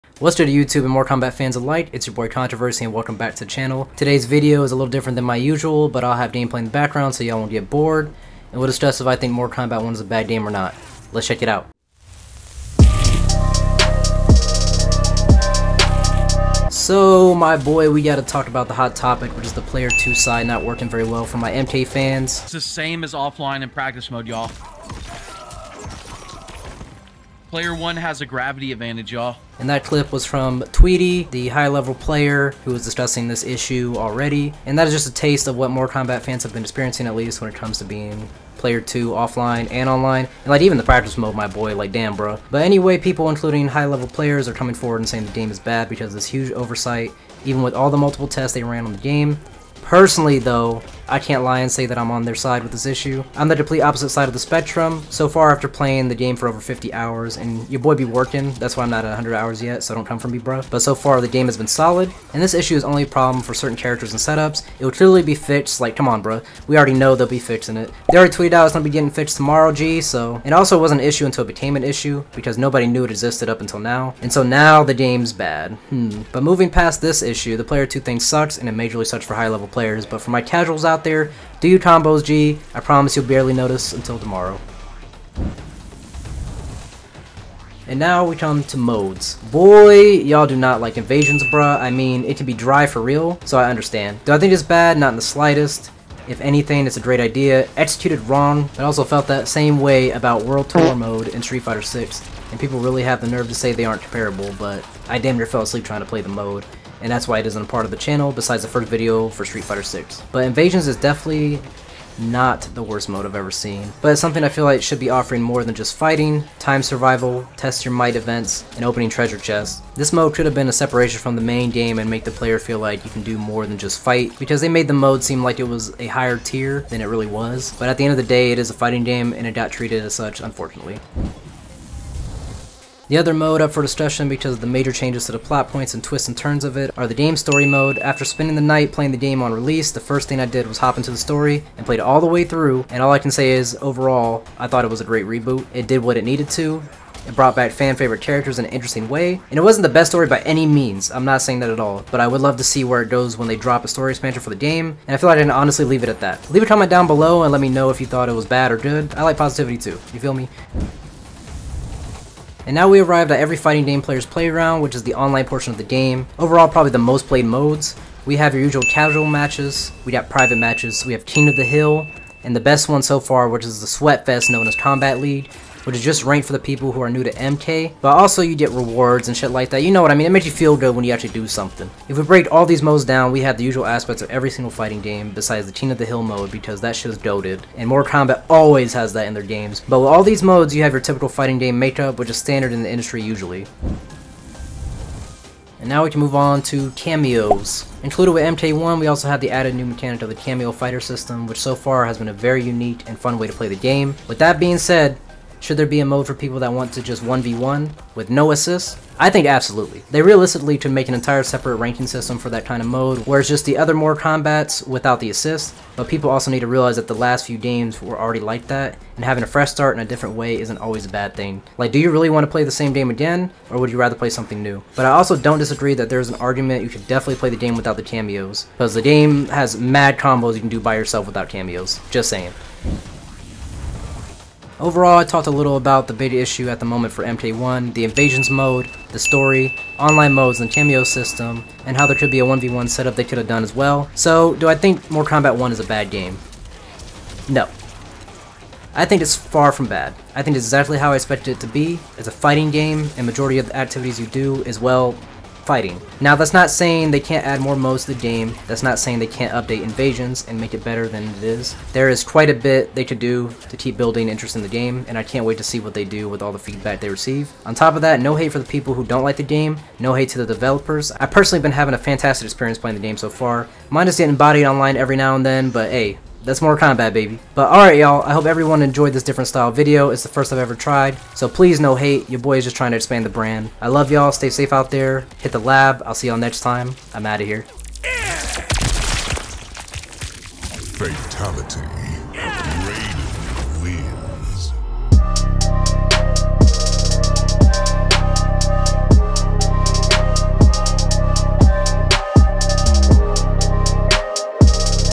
IS IT ACTUALLY BAD?... | Mortal Kombat 1 (Review)
What's good everyone! It's your boy ControverC, back with another Mortal Kombat 1 video. This time though, I am addressing a few things, and this is a first on the channel for a video like this. It's not the longest video, but it's a reasonable amount of time for someone to enjoy, and hear my thoughts on if I think Mortal Kombat 1 is a bad game!

DO YOU EVEN USE SOCIAL MEDIA?!